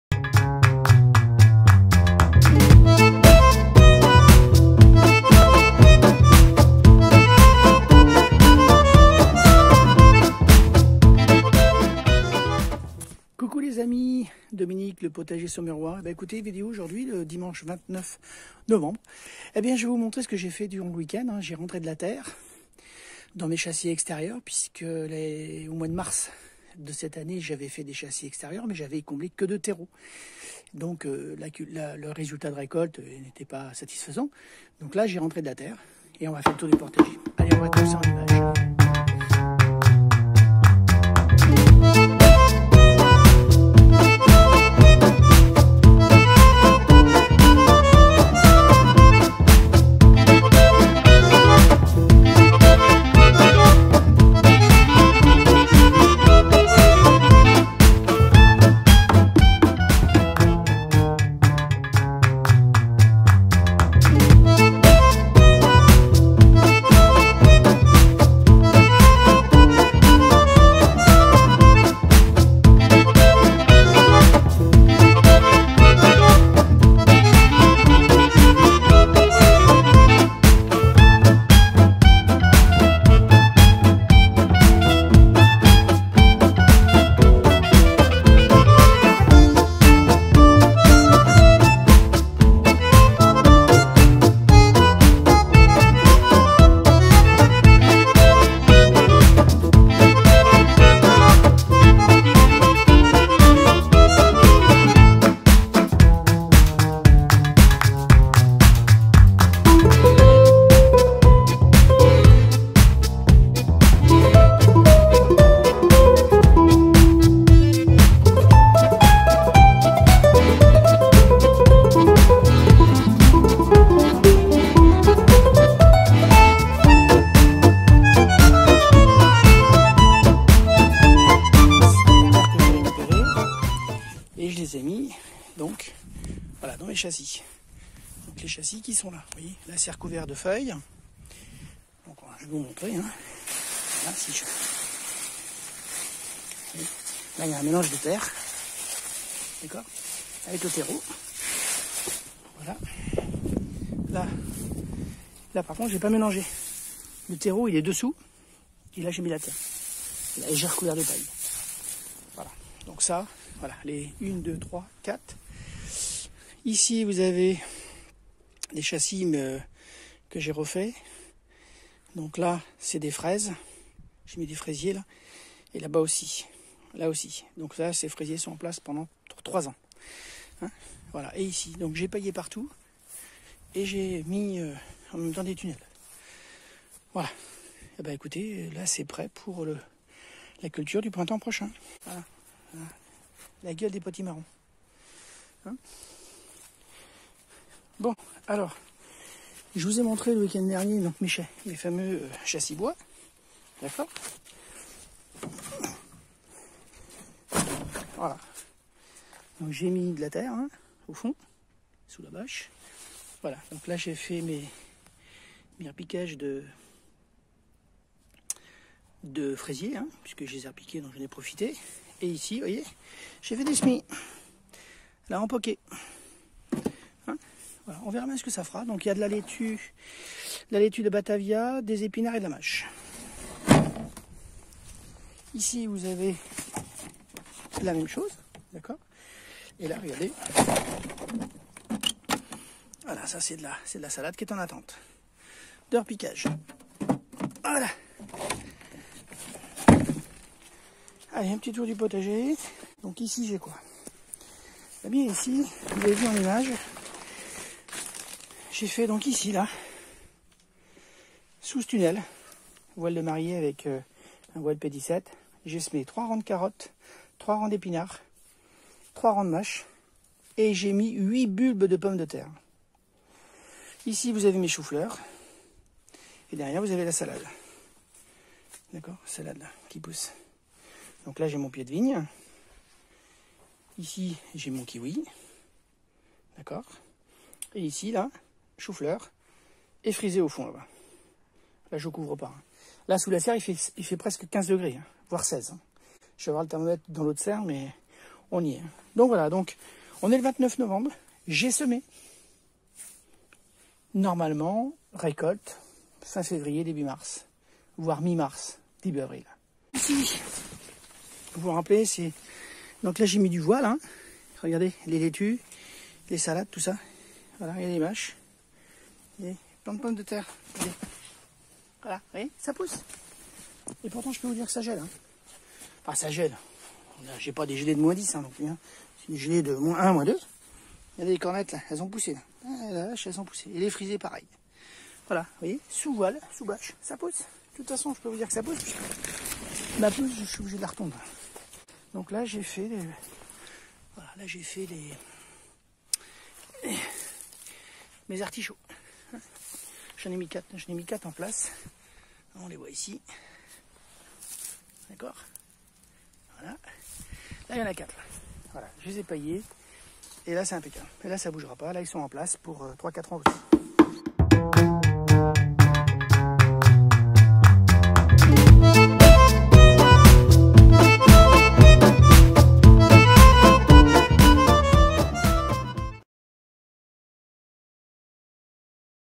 Le potager Saumurois semis en novembre 2020
Tour du potager - semis en novembre carotte, mache, épinards, pommes de terre, bouture de framboisier